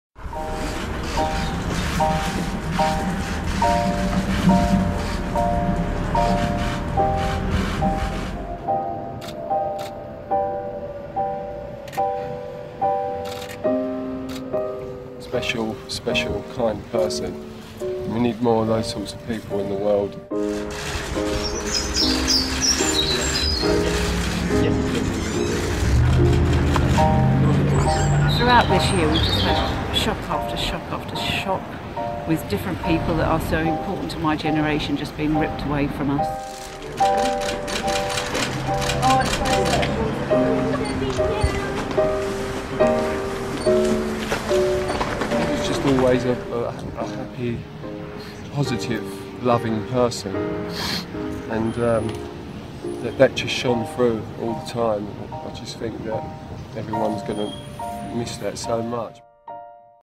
Mourners pay tribute to George Michael - Daily Mail
Fans and friends have paid tribute to the 53-year-old musician after he died from suspected heart failure.